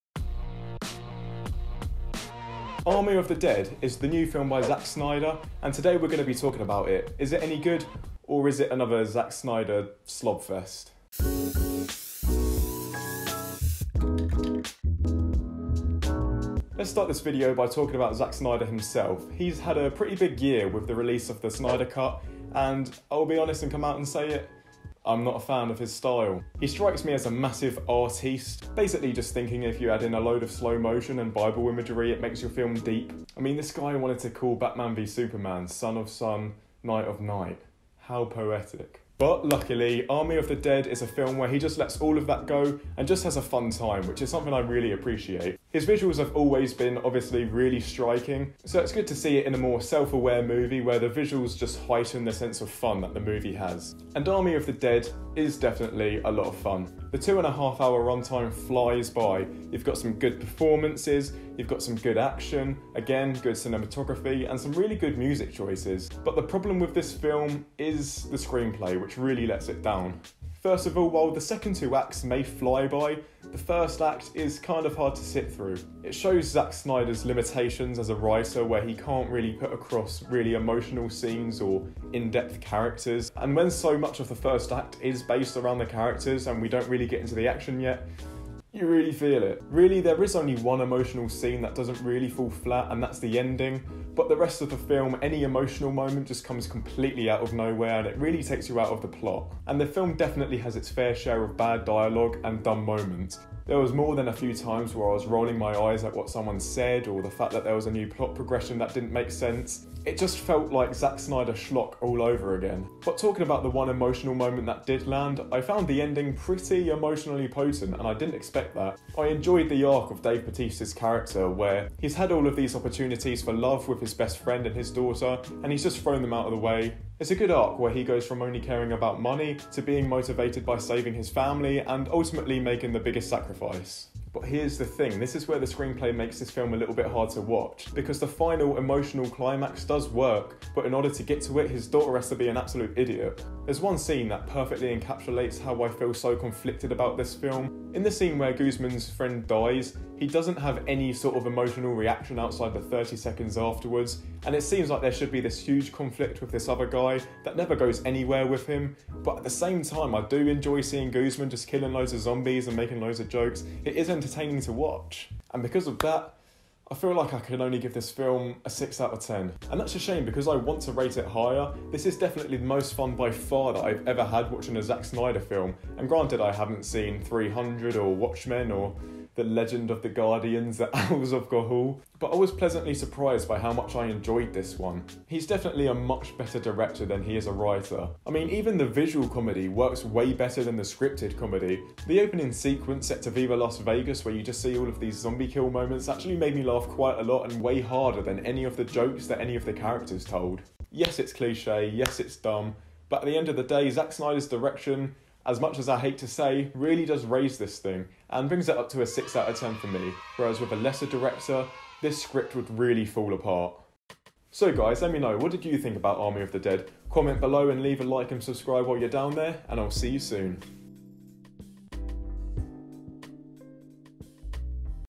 ARMY OF THE DEAD (2021) - QuickFire Review
Zack Snyder's second movie of 2021, ARMY OF THE DEAD, has just hit Netflix this week, so what do I think of it? Stick around and find out...

Army of the dead army of the dead review army of the dead spoiler review dave bautista zack snyder zombie movie